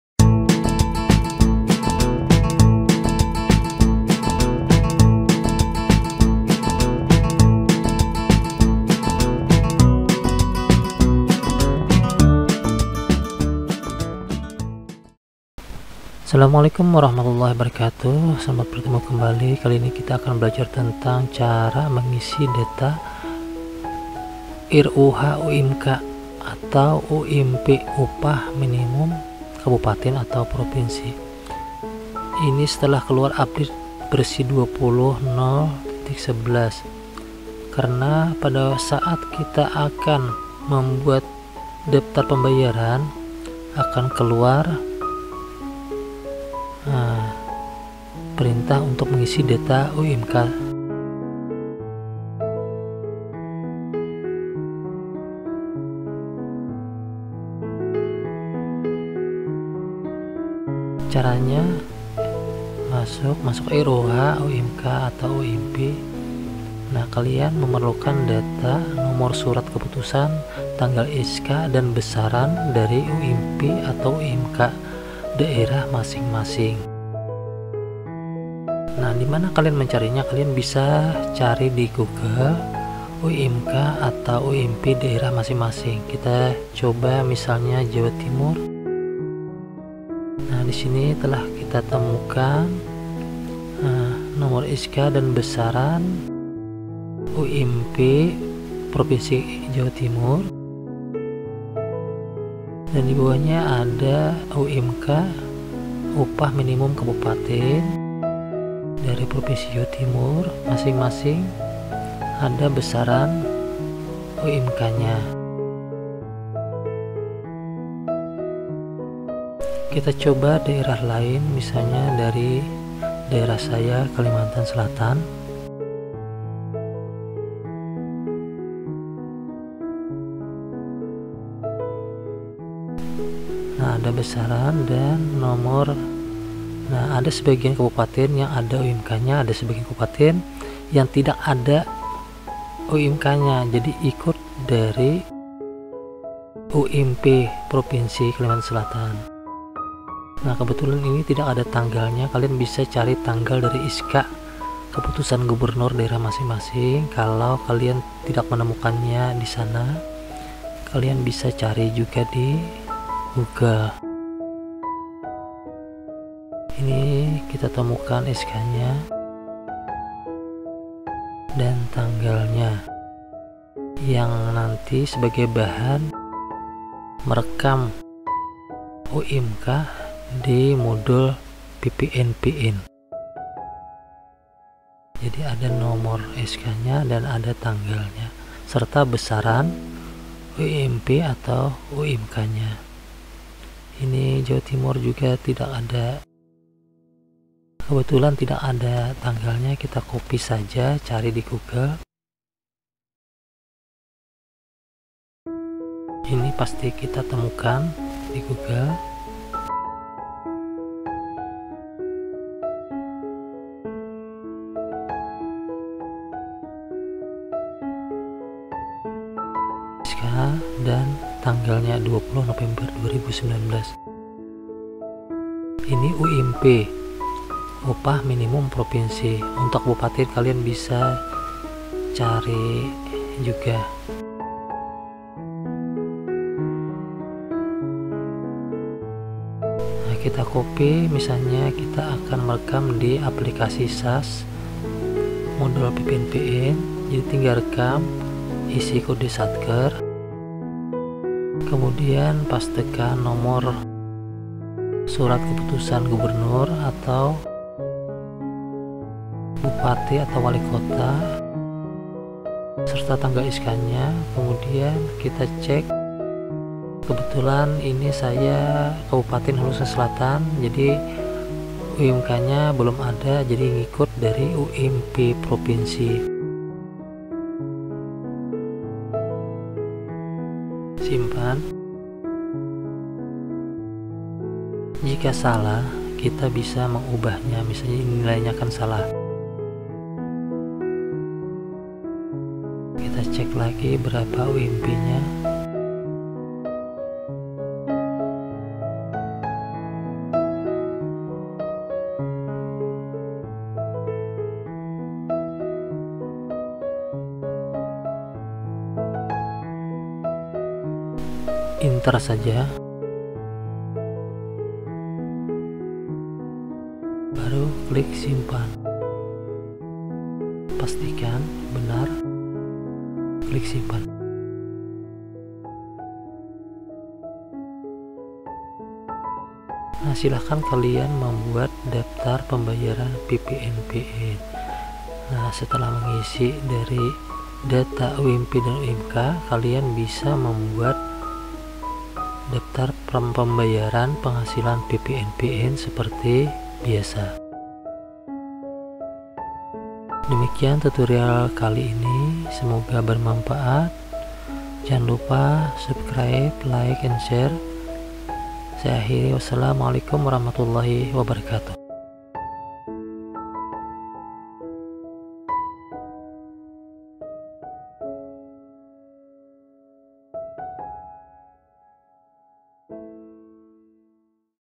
Cara Rekam UMK/UMP pada Modul PPNPN Aplikasi SAS20.0.11
Pada aplikasi SAS versi ini mulai diterapkan batas bawah sebagai dasar perhitungan iuran jaminan kesehatan adalah Upah Minimum Kabupaten/Kota. Jika tidak ada, maka menggunakan Upah Minimum Propinsi (UMP). Pengisian UMK/UMP dilakukan oleh satker masing-masing.
Penerapan Pemotongan :
a. Jika Pegawai disetting aktif BPJS, maka perlakuan pemotongan iuran Jamkes sebagai berikut :
- Jika Penghasilan dibawah atau sama dengan UMK, maka hitungan 1 % dari UMK
- Jika Penghasilan diatas UMK, maka hitungan 1 % dari penghasilan, dengan maksimal penghasilan sebesar Rp.12.000.000. Dengan kata lain jumlah maksimal potongan adalah Rp. 120.000,-
b. Jika pegawai disetting aktif non BPJS, maka tidak akan dipotong iuran jamkes berapapun penghasilannya (bukan peserta iuran jamkes PPNPN).